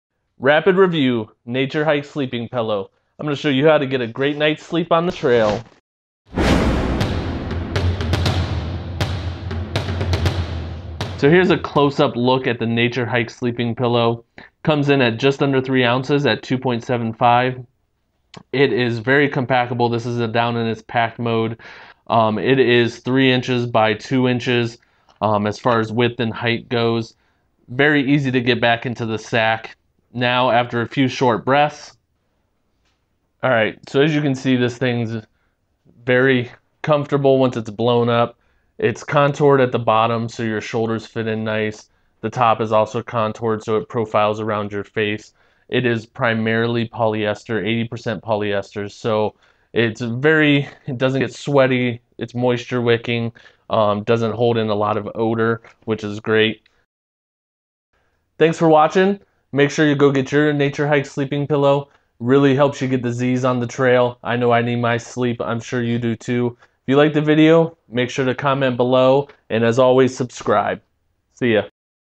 Naturehike Inflatable Camping Pillow | Backpacking, Hiking, Camping | Budget Backpacking Gear
Looking for budget backpacking gear? Look no further then the Naturehike Backpacking Pillow. This thing packs down small but packs a punch once it is inflated. This pillow is made of great material that doesn't absorb odor but maintains its shape well to give you a good nights sleep. You cant beat this thing, especially compared to that bag of dirty clothes that you are using now. This is a great piece of lightweight budget gear for camping or backpacking

Specs:
Weight - 2.75oz / 78g
Packed Size - 2.4 x 3 x 4.85 inches
Inflated Size - 17.71 X 11.41 X 4.33 inches